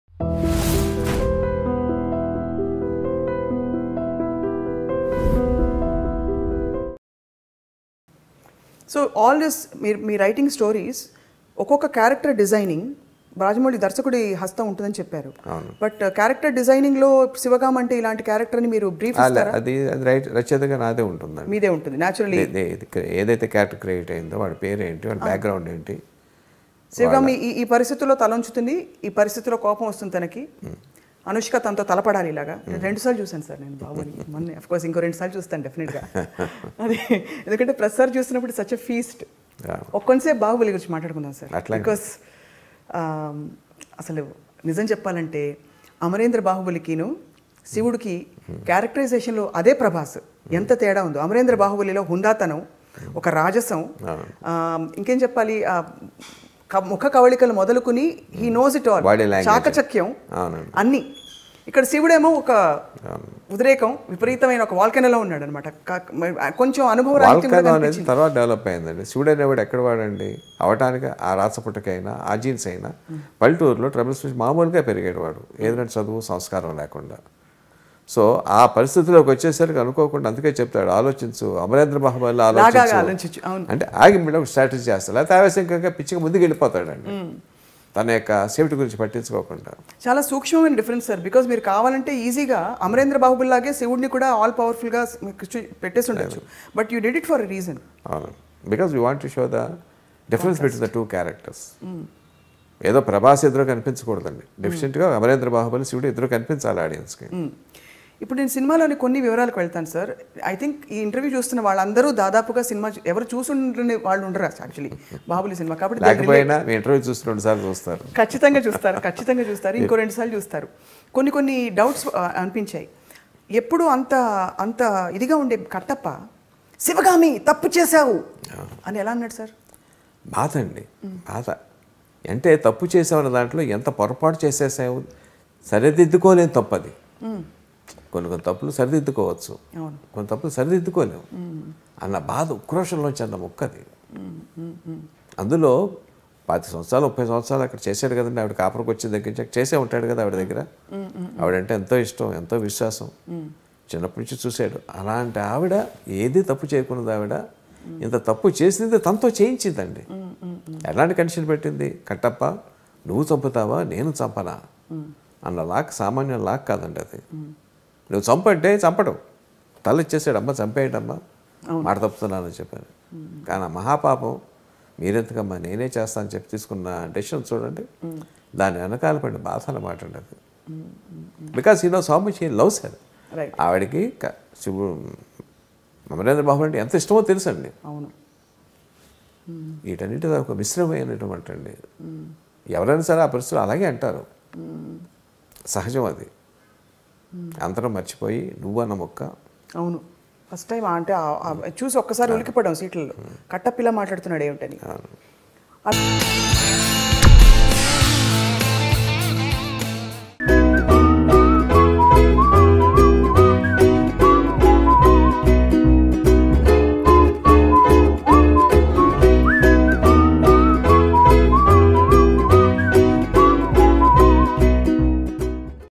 KV Vijayendra Prasad About Baahubali 2 Climax || Heart To Heart With Swapna
👉 Episode 1 K. V. Vijayendra Prasad is an Indian film screen writer and director known for his works in Telugu cinema and Bollywood. In 2011, he directed Telugu movie Rajanna which won the Nandi Award for Best Feature Film. Since 1988, he was the screen writer for more than twenty five films, most of which turned out to be block busters. His works include Baahubali: The Beginning, Baahubali: The Conclusion and Bajrangi Bhaijaan which became biggest blockbusters.

He also has a rare record in Hindi cinema in two consecutive weeks, he has delivered (as story and co-screenplay writer) two of the biggest hits in history: his son’s directorial “Baahubali — The Beginning” and “Bajrangi Bhaijaan.”

In this exclusive interview, he had shared many interesting things about his film career and many personal things!